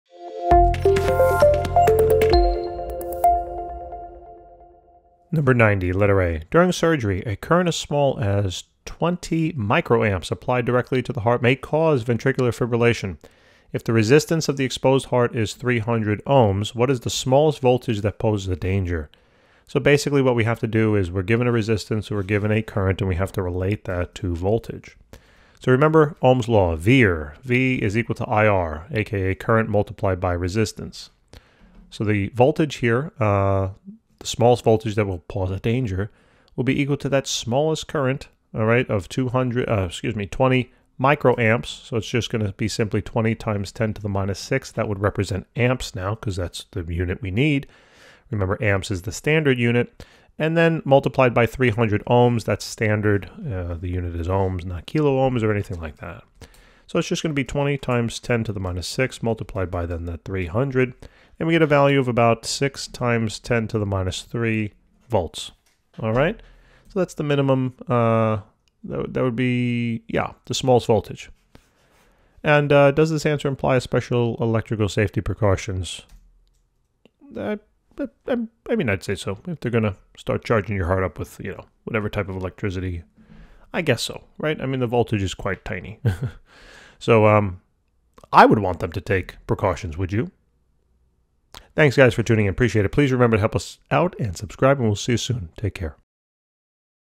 20.90 | During surgery, a current as small as 20.0 μA applied directly to the heart may cause
(a) During surgery, a current as small as 20.0 μA applied directly to the heart may cause ventricular fibrillation. If the resistance of the exposed heart is 300 Ω, what is the smallest voltage that poses this danger? (b) Does your answer imply that special electrical safety precautions are needed?